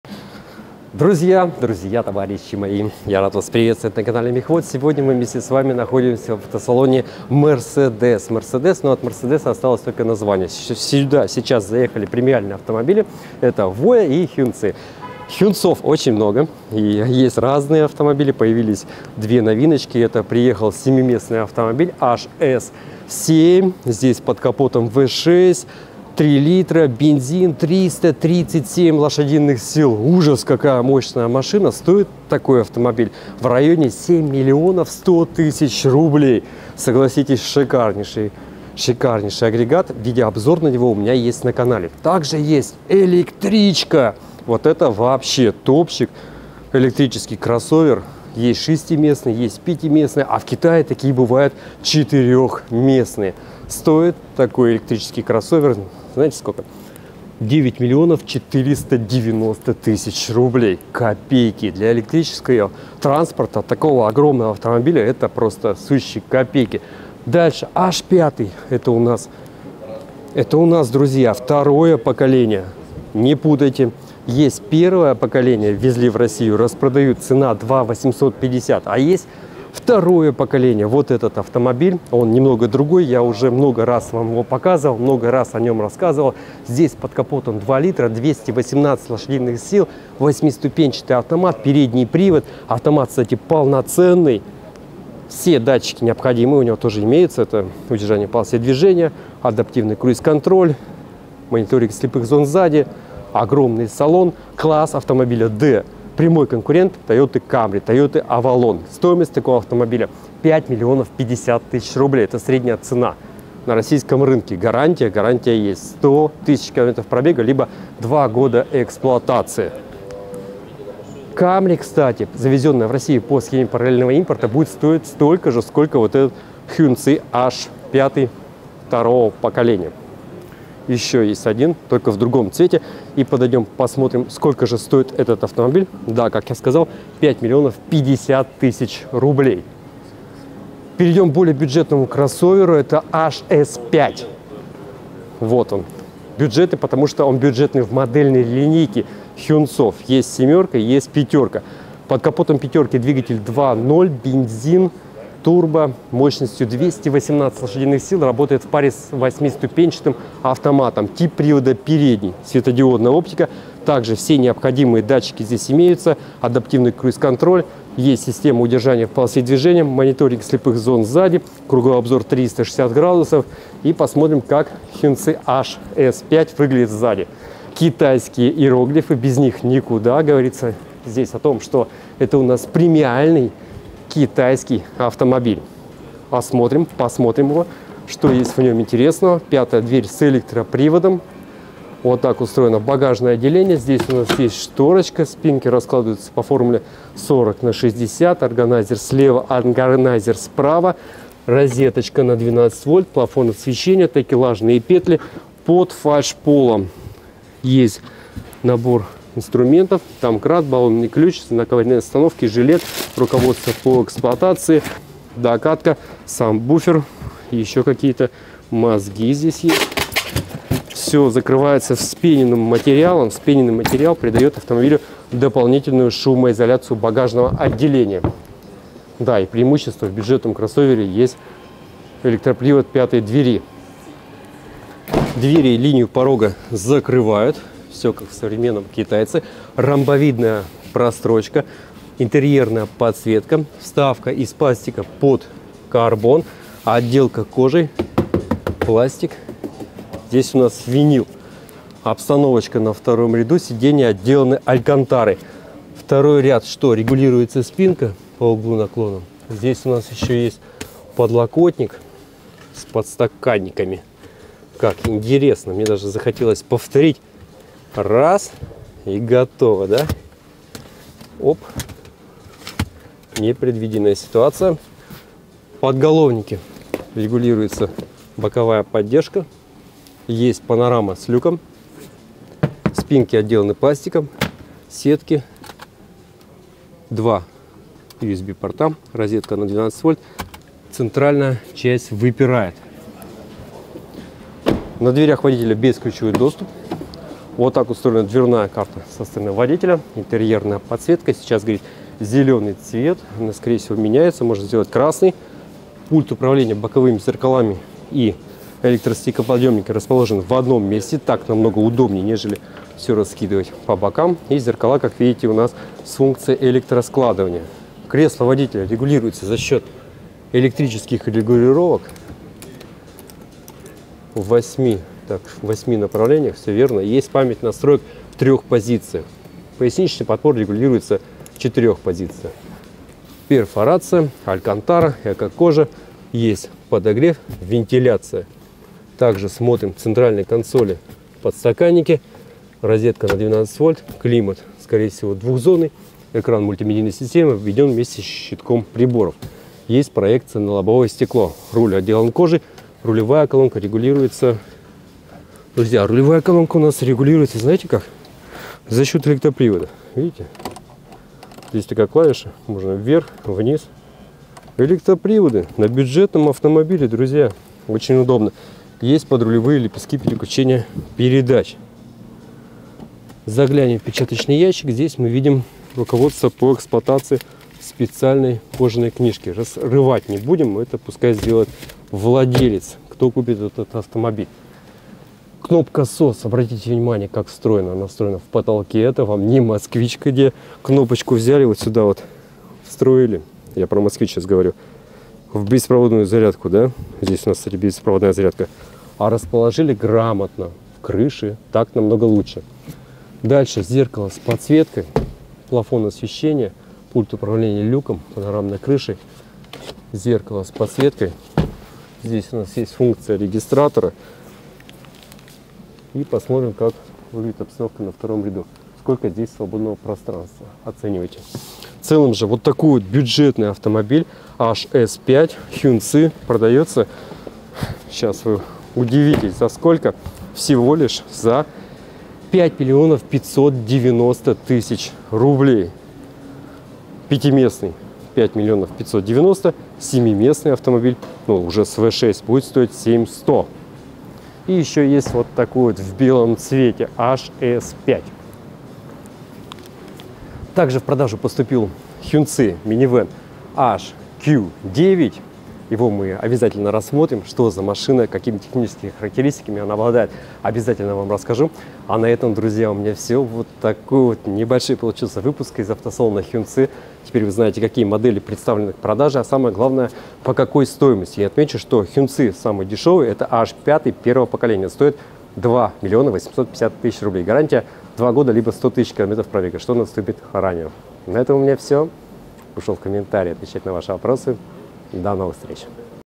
Нас упорно хотят пересадить на "Хончи", а мы всё думаем, что лучше купить"LADA"
В дилерском центре "Hongqi" посмотрел какие модели авто представлены в продаже на российском рынке, а так же узнал их стоимость.
Hongqi (Хунцы) правильно произносить "Хончи", переводится как "Красное знамя" — китайская автомобильная марка премиального сегмента, подразделения компании FAW Group, официально на рынке России с 2023 года.
В шоу-руме автосалона "Hongqi" были представлены следующие модели: электричка E-HS9, семиместный кроссовер HS7, пятиместный кроссовер HS5, седан H5 (перове поколение). На улице: седан H5 (второе поколение), седан H9 и минивэн HQ9.
Вот такая, друзья, модельная линейка доступна на российском рынке. Что думаете по этому поводу, пишите в комментариях.